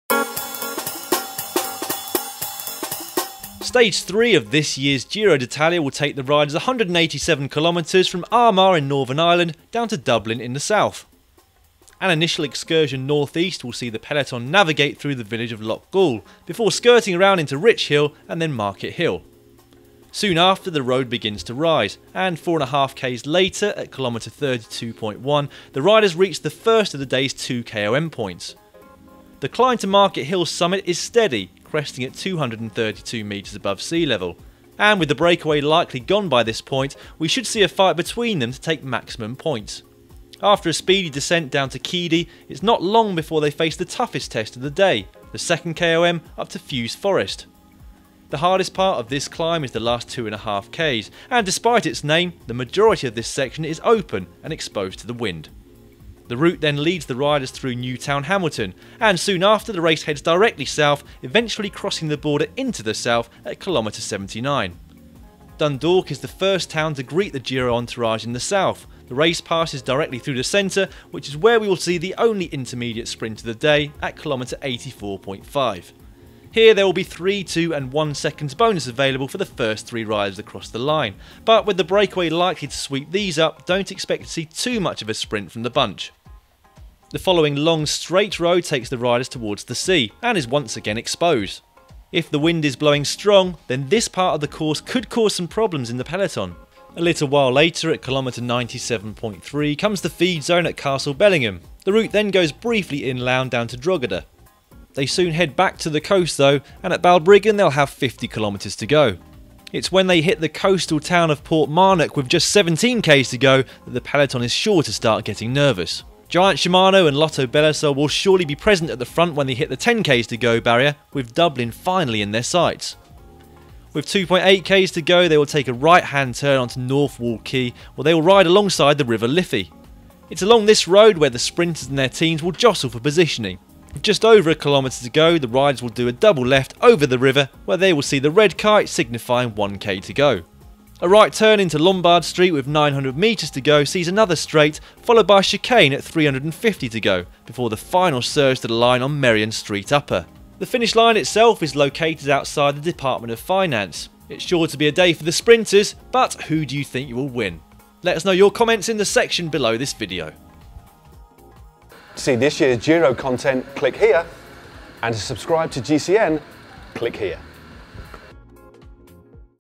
Giro d'Italia 2014 Stage 3 Fly Through
Stage three of the Giro d'Italia 2014 travels 187km from Armagh to Dublin.

GCN's stage previews are Google Earth fly throughs of key stages of the 2014 Giro, giving you a detailed look at the route, the terrain, and highlighting all of the king of the mountain (KOM) and sprint points along the route, along with insights from GCN's ex pro and 2010 Tour de France finisher, Daniel Lloyd.